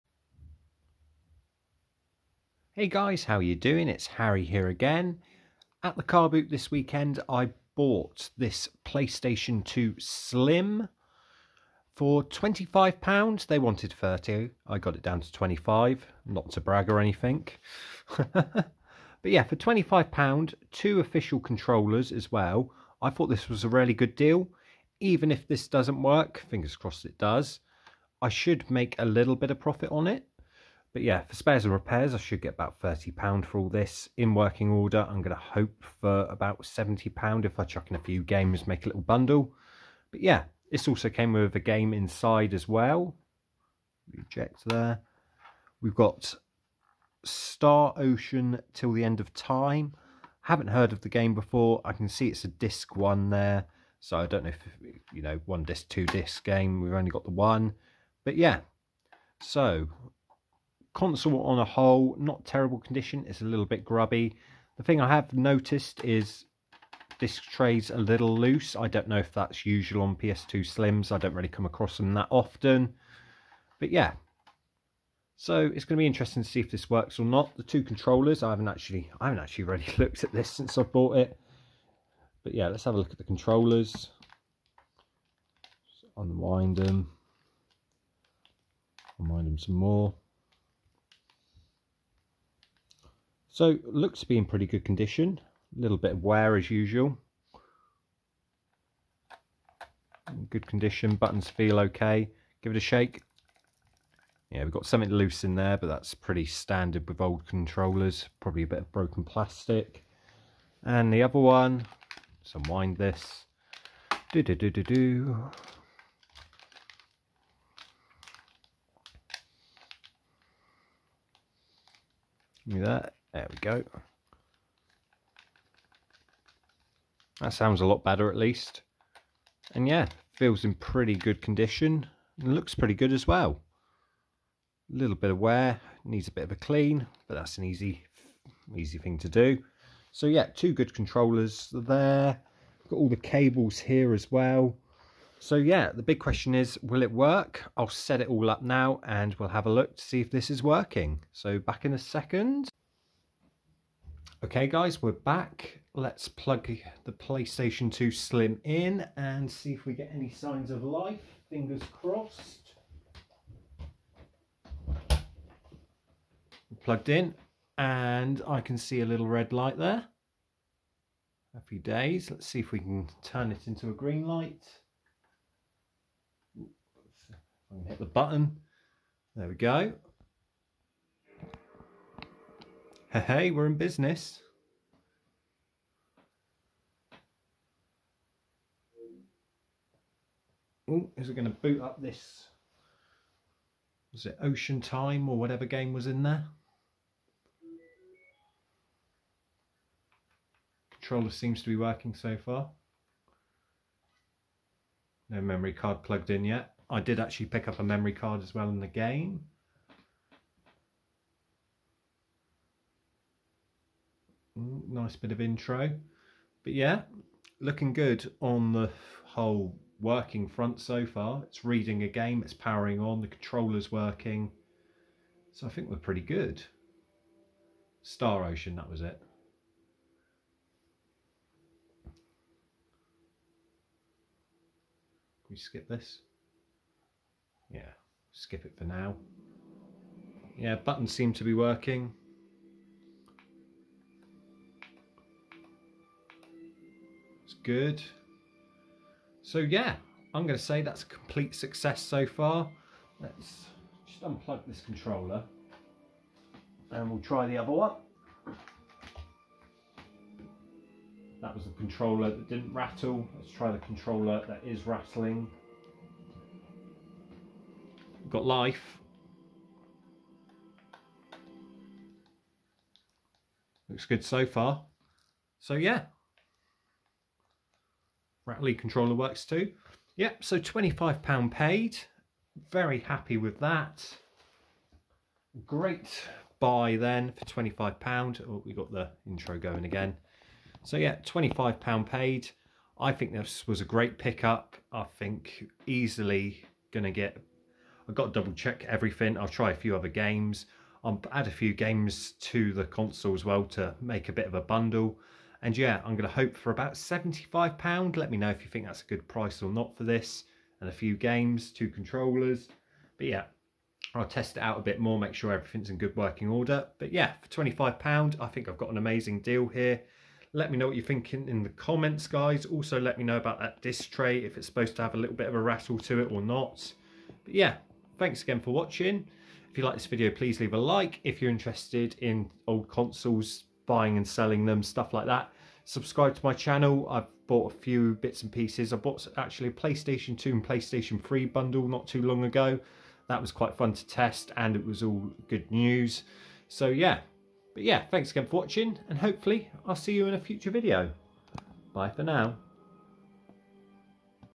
This playstation 2 cost me £25 was it worth buying?
I think it got a good deal on this ps2 slim let me know what you think in the comments. also sorry for the quality of the video towards the end not got the best set up. thanks for watching and remember to subscribe.